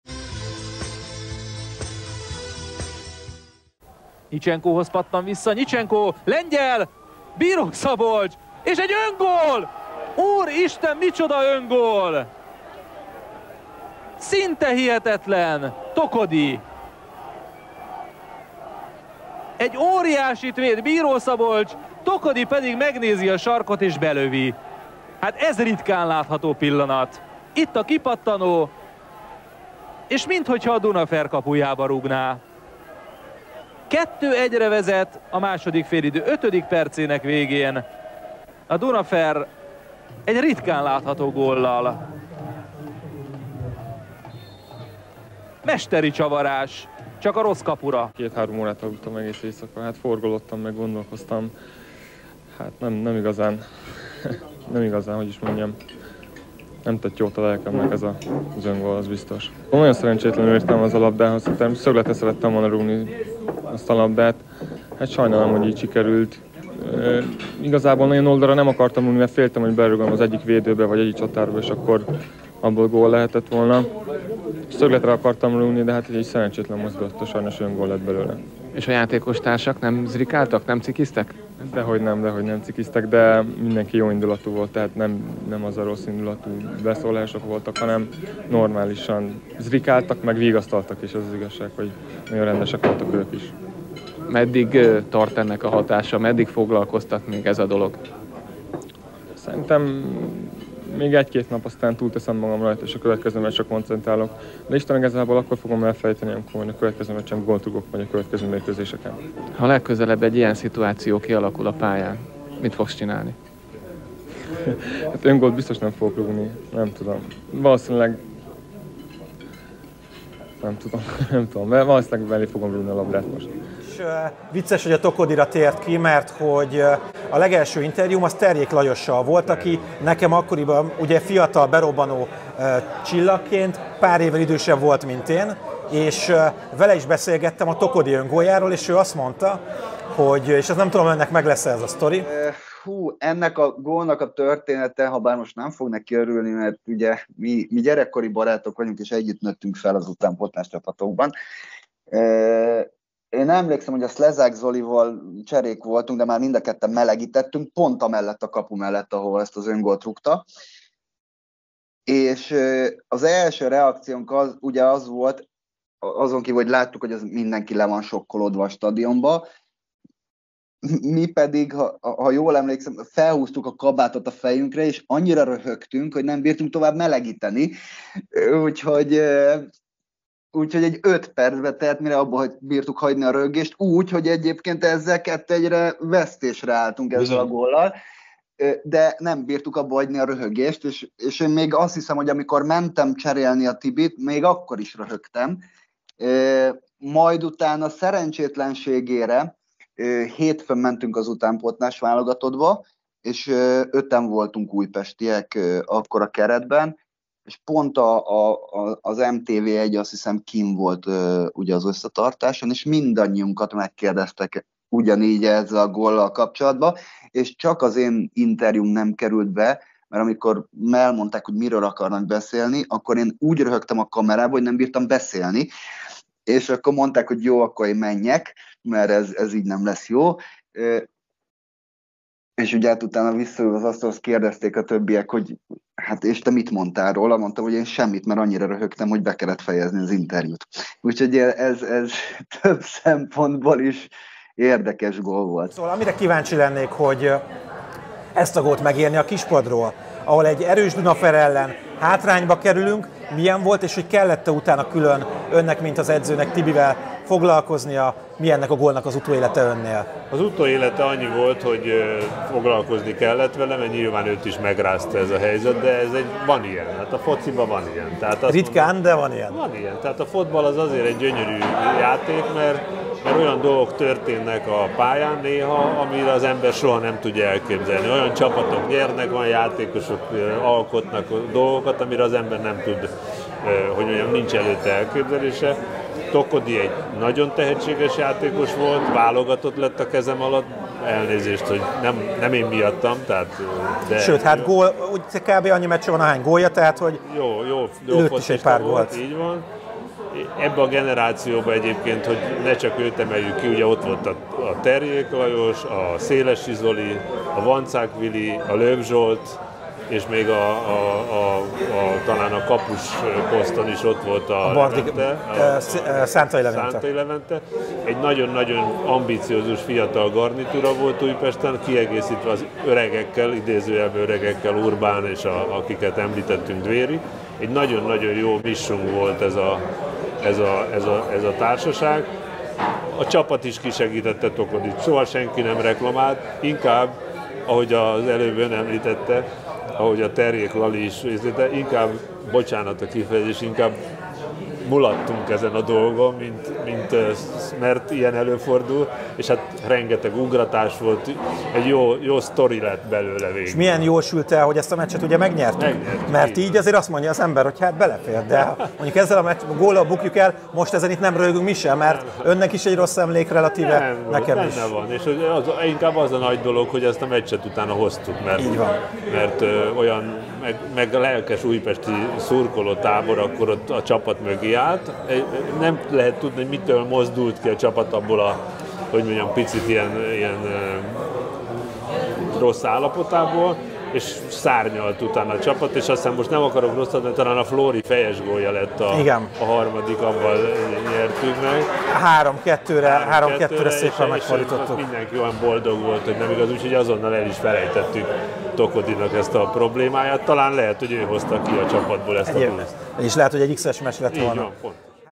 A legendás Tokody öngól titkai
Tokody Tibi mellett csapattársa, Terjék Lajos és az akkori edzőjük, Kisteleki István mondja el ennek a 2001. április 21-ei öngólnak a hátterét, titkait.

Bíró Szabolcs, az Újpest akkori kapusa a következőket mesélte el a Tokody öngólról :
⚽az 100%-ban kizárható, hogy bunda, vagy bármi hasonló miatt direkt lőtt volna öngólt Tibi. Egyrészt nem olyan srác volt, másrészt mert a pályán az arca, a testbeszéde ezt teljesen kizárja. Teljesen összeomlott az eset után Tibi
⚽nagyon magába volt fordulva és össze volt törve Tibi a meccs után, az öltözőben
⚽Szabi szerint is szögletre akarta rúgni a labdát Tibi
⚽nem hiszi, hogy megzavarodott volna Tibi és direkt lőtte a kapuba a labdát, mert azt hitte, hogy a másik kapura lő. Semmi nem indokolta volna, hogy zavart legyen
⚽Urbán Flórival nézett össze először Szabi a gól után, és Flóri már a pályán kérdezte Tibitől, hogy "Mi volt a terv, Tibi? :D"
⚽ilyen öngólt azóta se látott Szabi, hogy egy játékos leveszi, megállítja, benézi a sarkot, és belövi a saját kapujába a labdát :)
⚽évente legalább 4-szer feljön ez a Tokody öngól Szabinál, vagy újságíró, vagy szurkolók, vagy a barátai hozzák fel a témát
⚽Szabi egy jó emlékként, jó poénkét gondol vissza erre most már, de ott a pályán azért zavarta a dolog :)

A teljes interjú Kisteleki Istvánnal a Hamis Kilences Youtube csatornán jelent meg 2022-ben, itt tudjátok megnézni